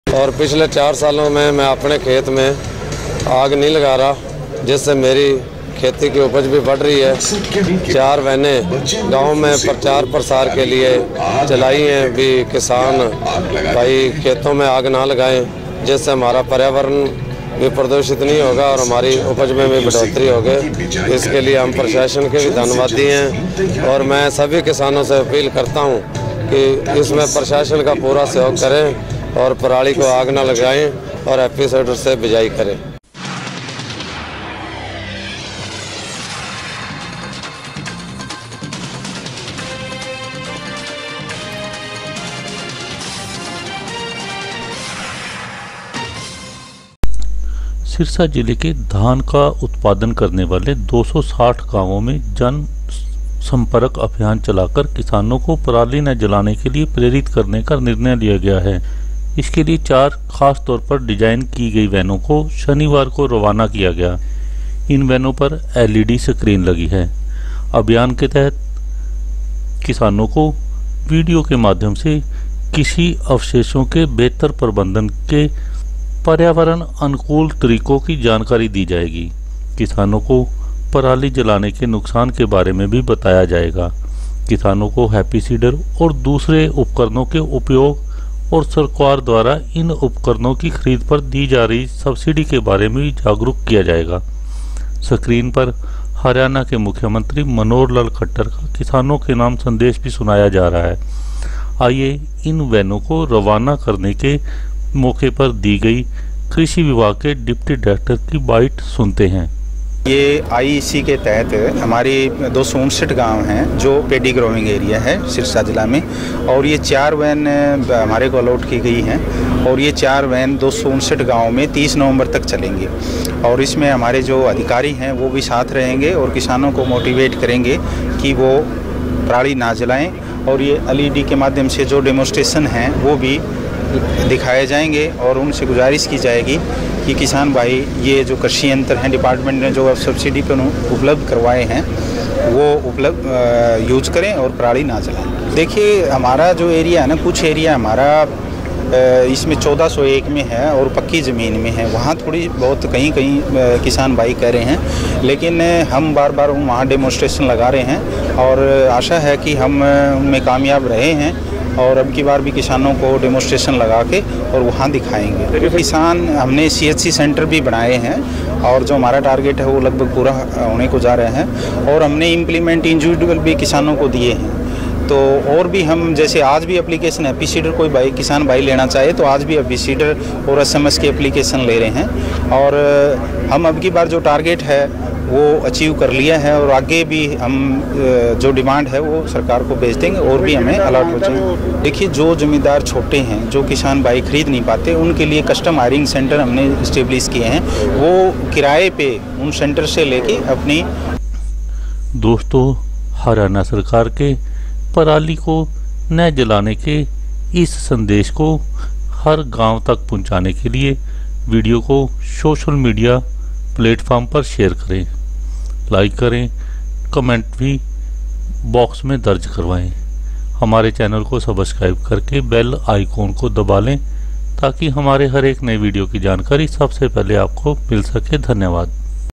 Sirsa District / Intensive Education Campaign Against Stubble Burning In Sirsa District
Intensive Education Campaign Against Stubble Burning In Sirsa District

सिरसा जिले के किसानों को पराली न जलाने को प्रेरित करने निकली चार खास गाड़ियां

This video is about agriculture department Haryana launched a special Intensive Education Campaign in Sirsa district against stubble burning. Four special vehicle were flagged off by deputy director agriculture Sirsa Babu Lal. These vehicle will cover all 260 paddy growing villages of Sirsa district. Farmers will be educated
about harms of stubble burning.